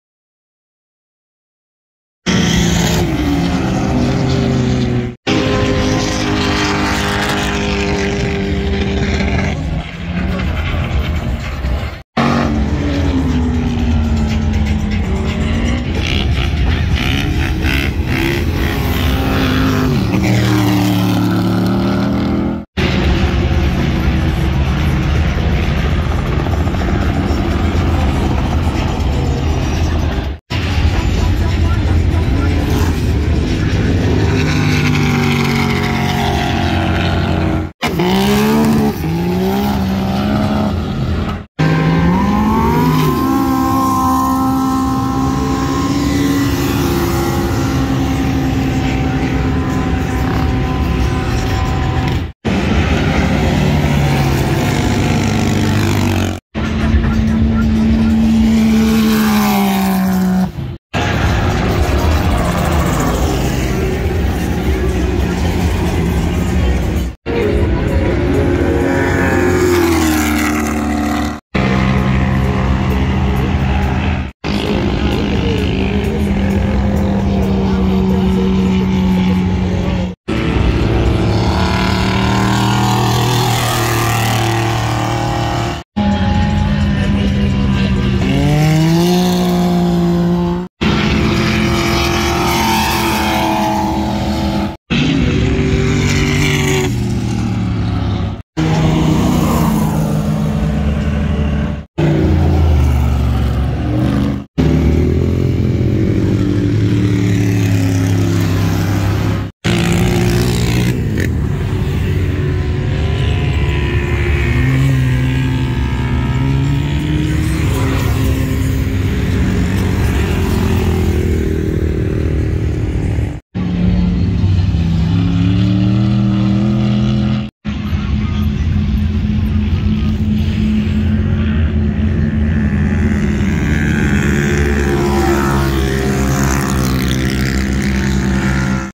GLAMIS | HALLOWEEN | 2024 | CAMP RZR | FRIDAY DRAGS
FRIDAY EVENING DRAGS OUT IN GLAMIS FOR HALLOWEEN/CAMP RZR 2024.